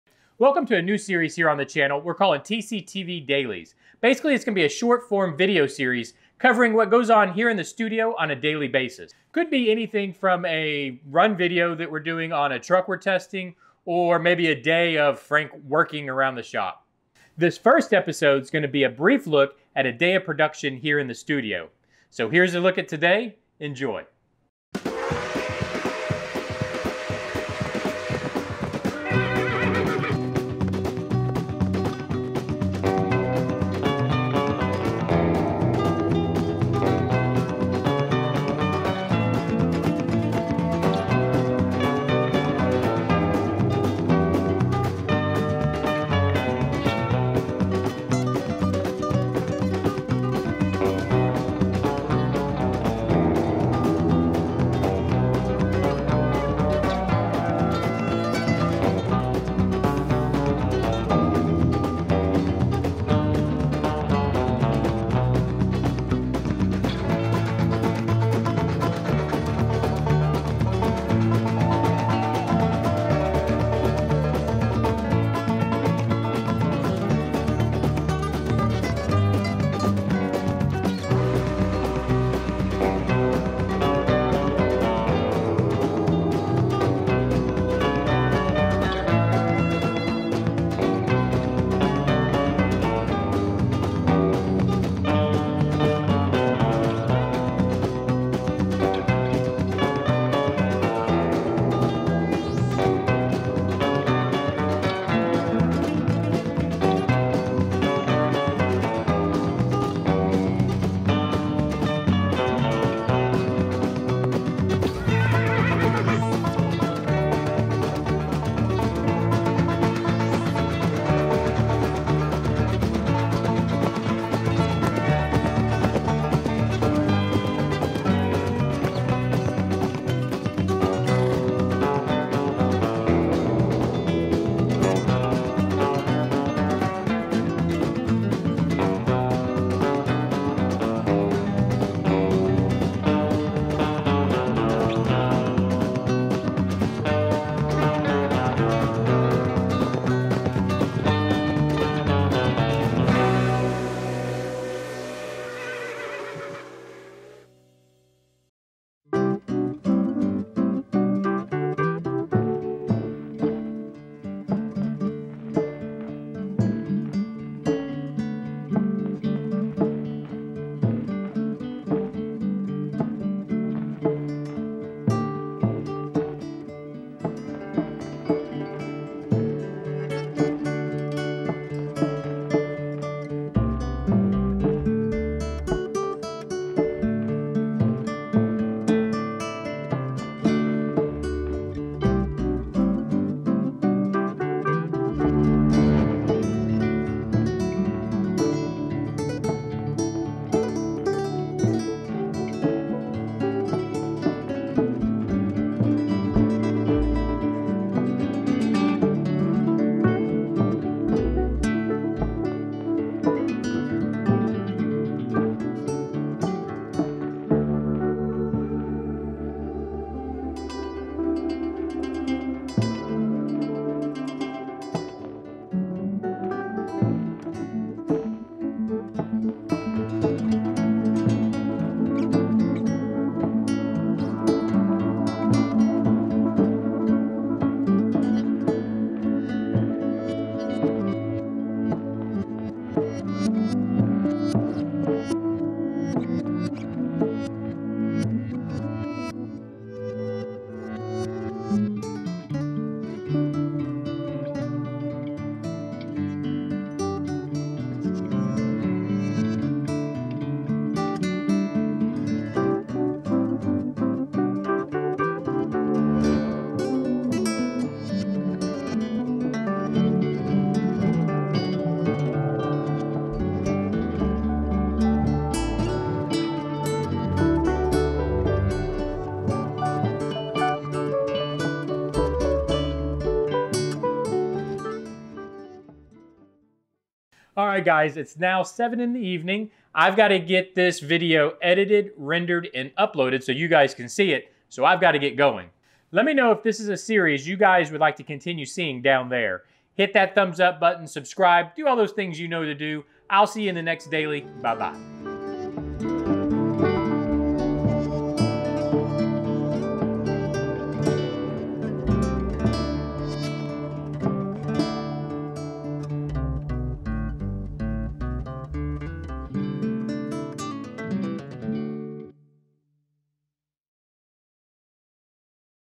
A Day of Production in the Studio! - TCTV Daily E1
In this first episode of TCTV Daily we take a quick look at what happens in one day at the studio!